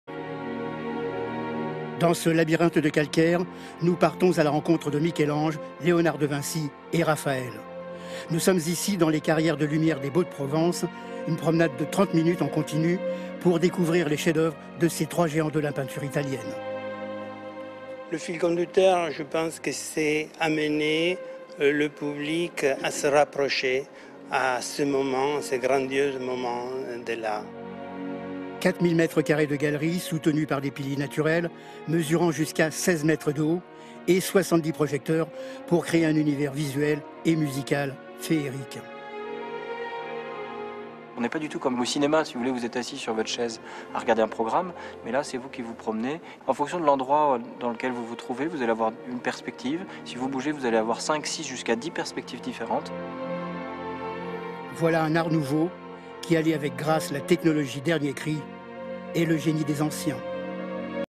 Quand la technologie se met au service de l'art de la Renaissance
Toute l'info :
Un spectacle propose de projeter des chefs-d'oeuvre de la Renaissance sur des parois de calcaire.